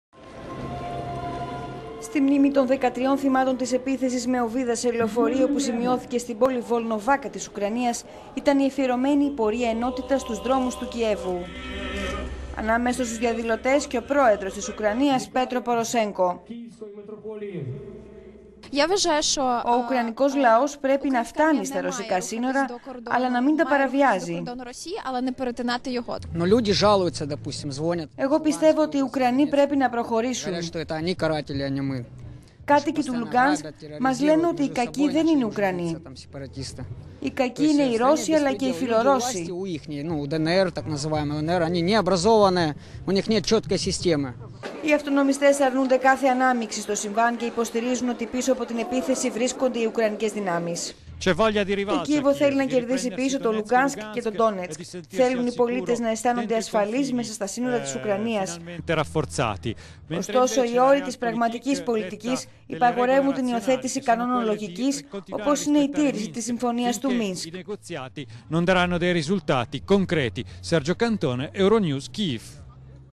Ουκρανία: Χιλιάδες διαδήλωσαν στη μνήμη των θυμάτων της επίθεσης στη Βολνοβάκα
Στη μνήμη των 13 θύματων της επίθεσης με οβίδα σε λεωφορείο, που σημειώθηκε στην πόλη Βολνοβάκα της Ουκρανίας, ήταν αφιερωμένη η πορεία ενότητας στους δρόμους του Κιέβου.

Ανάμεσα τους στην πλατεία Ανεξαρτησίας, βρέθηκε και ο πρόεδρος της χώρας, Πέτρο Ποροσένκο.

Οι αυτονομιστές αρνούνται κάθε ανάμειξη στο συμβάν και υποστηρίζουν ότι πίσω από την επίθεση βρίσκονται οι ουκρανικές δυνάμεις.…